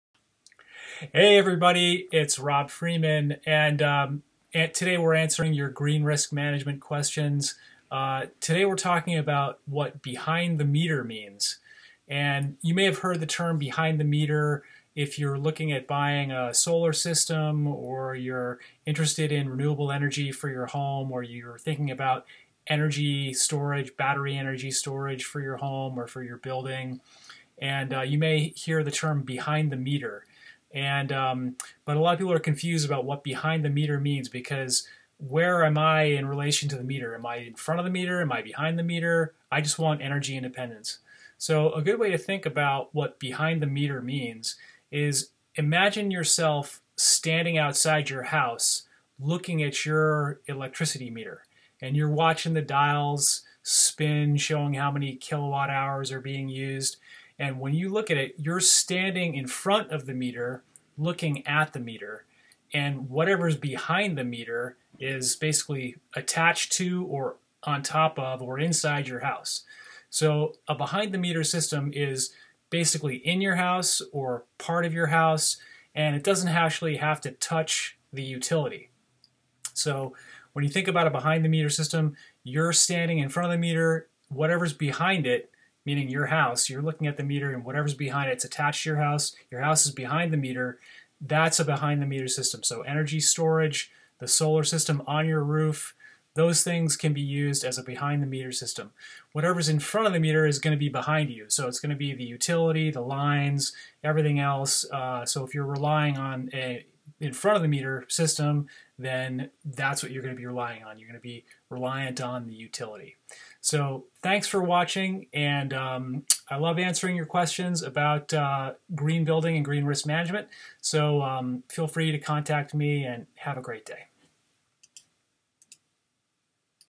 What Does "Behind the Meter" Mean?
- You may have heard the term "behind the meter" used in conversation when discussing solar and battery energy storage systems that can help you achieve energy independence...

But what does behind the meter mean? And what about "in front of the meter"? If you're confused about what these terms mean, this article describes a simple way to think about these increasingly common terms.